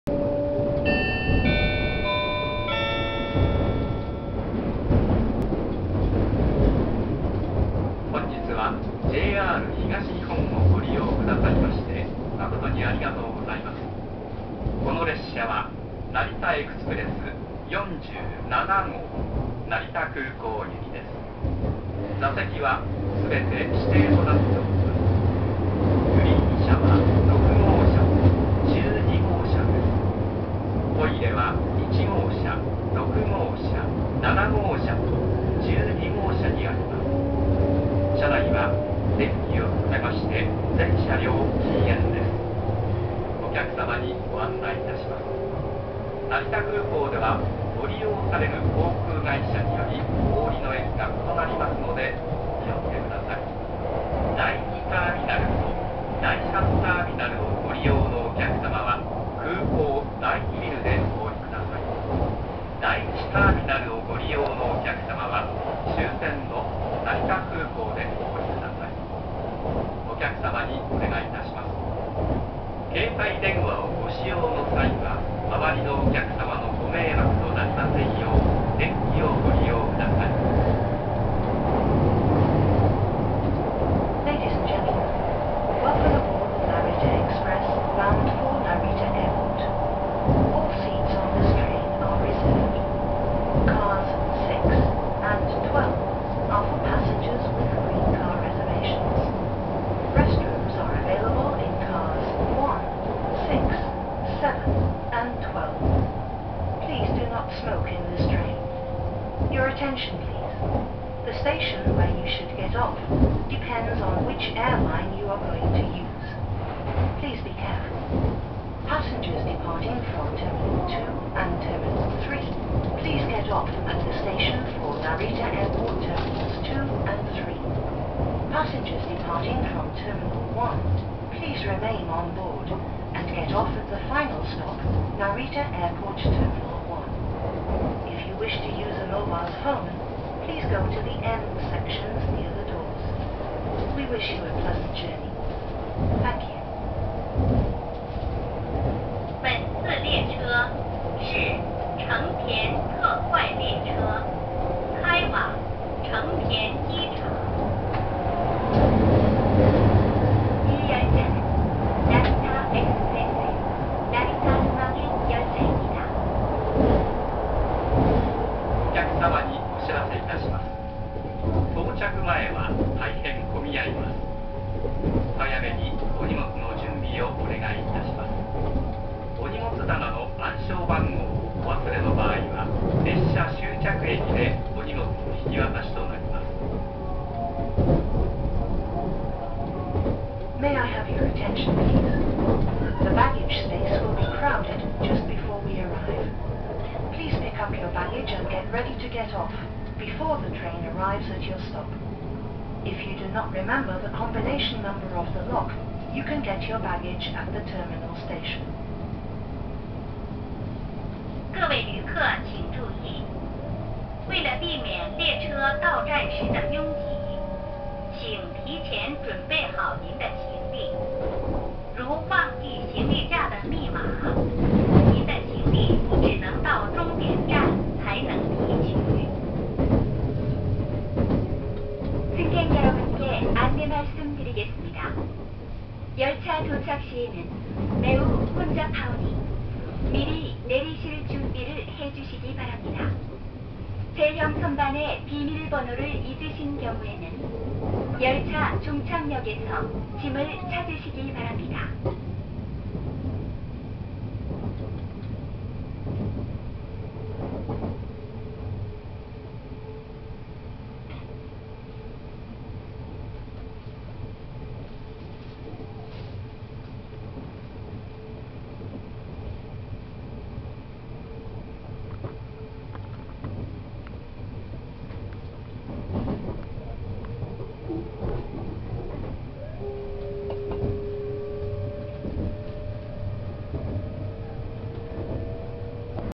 特急成田エクスプレス号成田空港行き 東京駅発車後四カ国後車内放送
特急成田エクスプレス号成田空港行きの東京駅発車後の車内放送です。日本語・英語・中国語・韓国語の順に流れます。